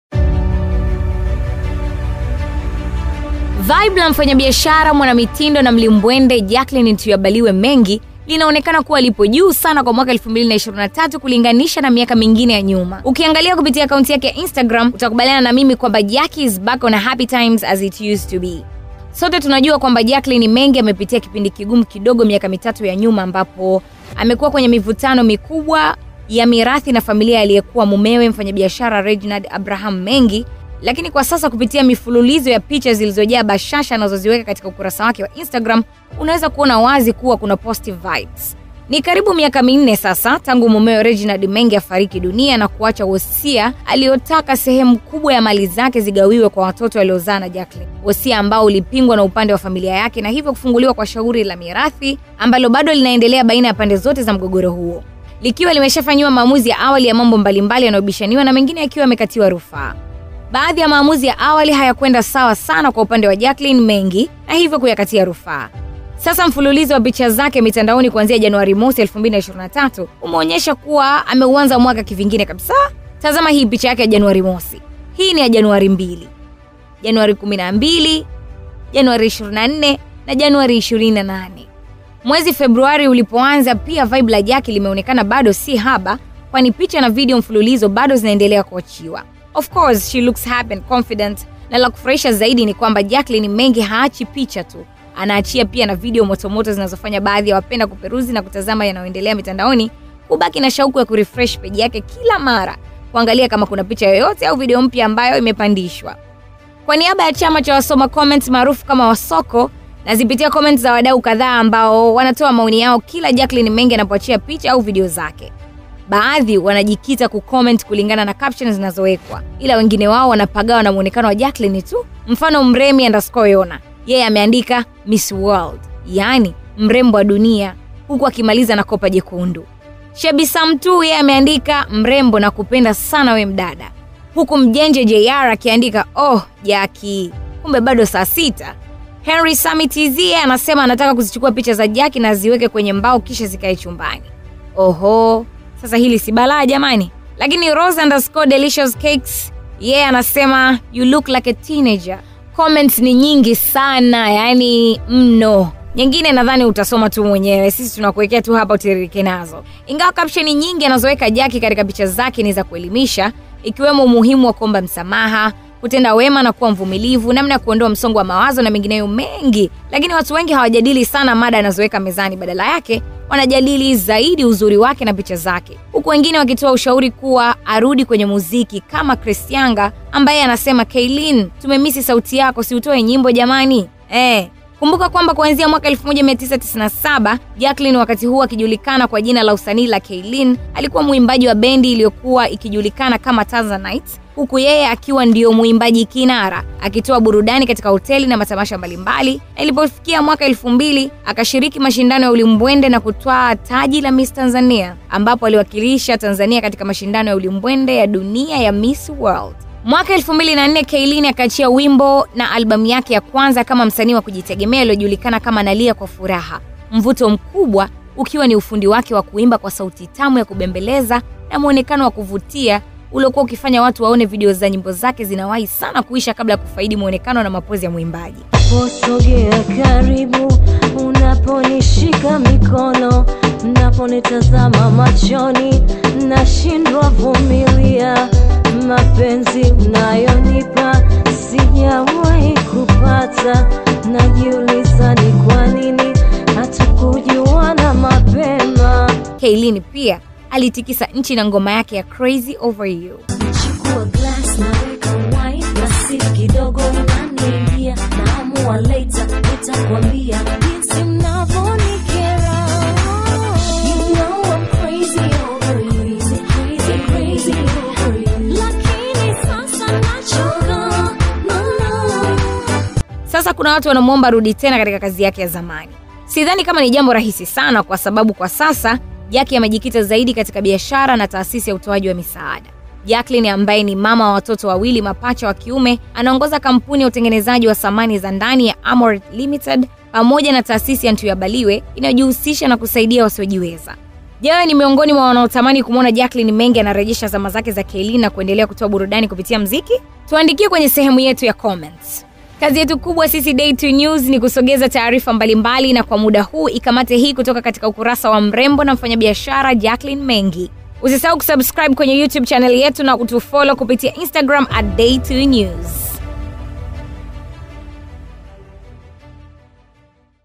VIPICHA VYA 'KICHOKOZI' VYA JACQUELINE MENGI VYAPAGAWISHA MITANDAONI/ "OOH JACKY KUMBE BADO....."
Mfanyabiashara Jacqueline Ntuyabaliwe Mengi ambaye ni Mjane wa Hayati Dk. Reginald Abraham Mengi, anaonekana kuuanza mwaka kwa vibe la juu sana.